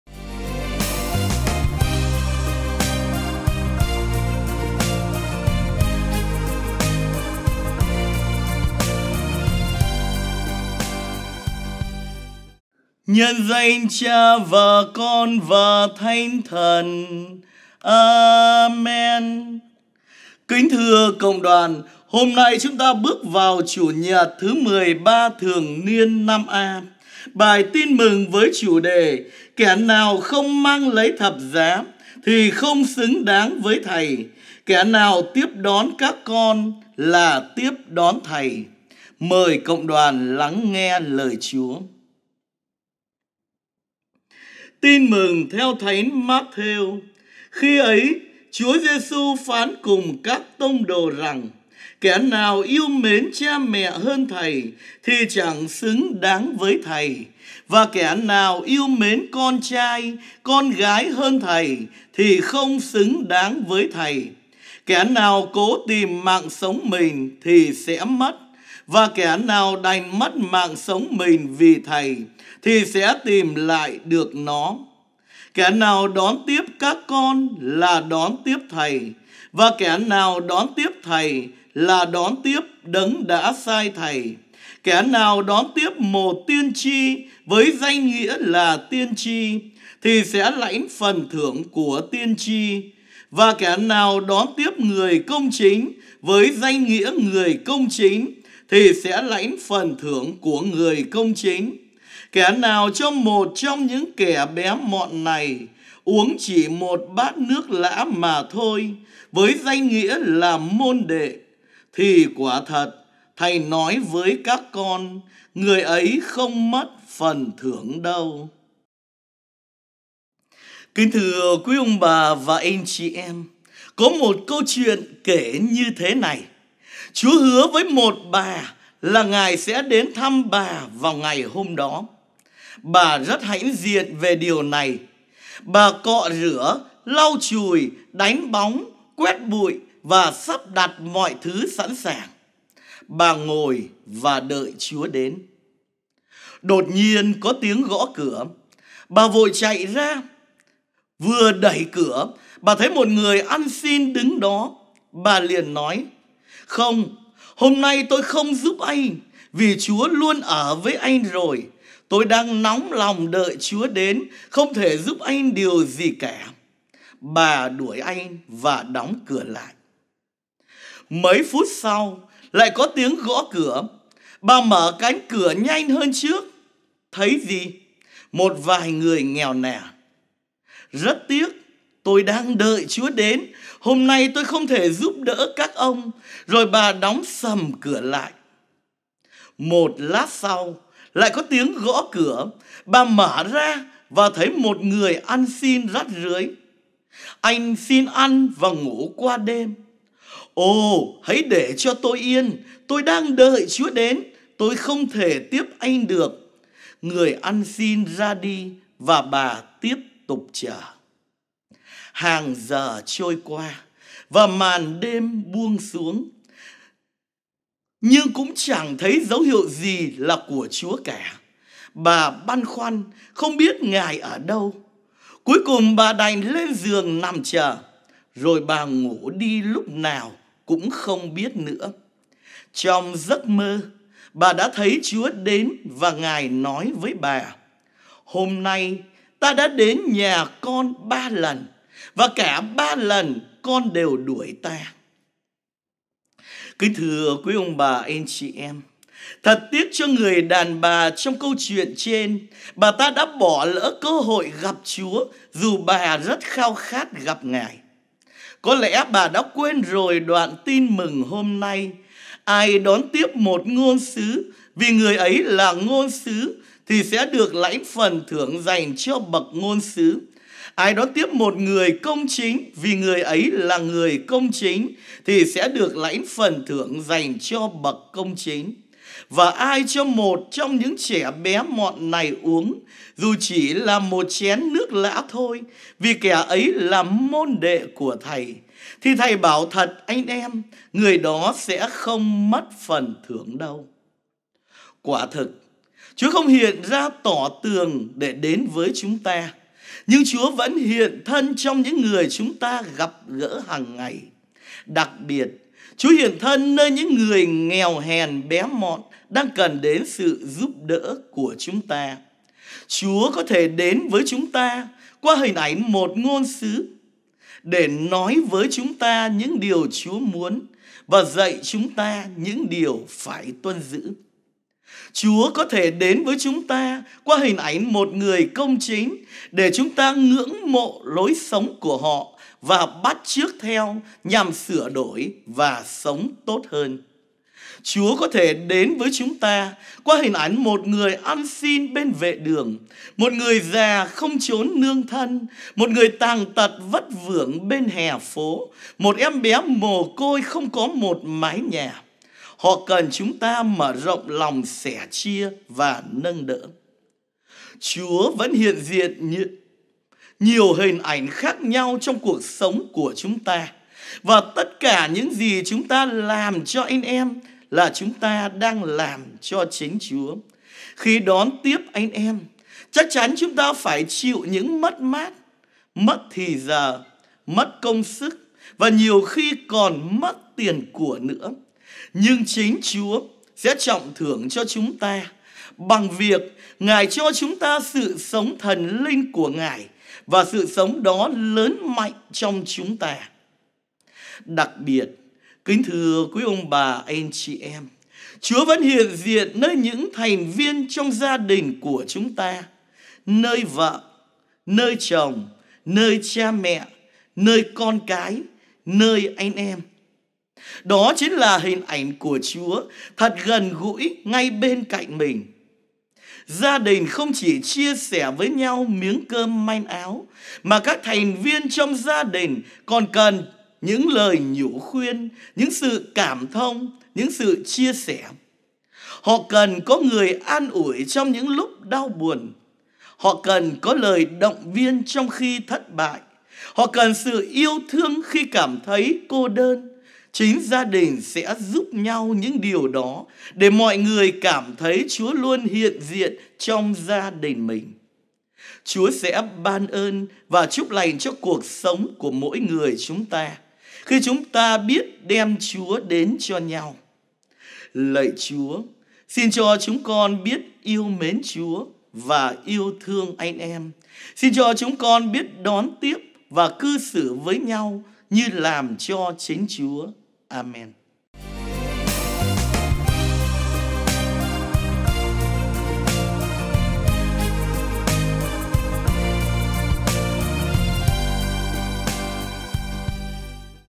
Muối Men Cho Đời 163: Bài Giảng Chúa Nhật XIII Thường Niên - Năm A
Lm. Vinh Sơn Nguyễn Tuấn Dương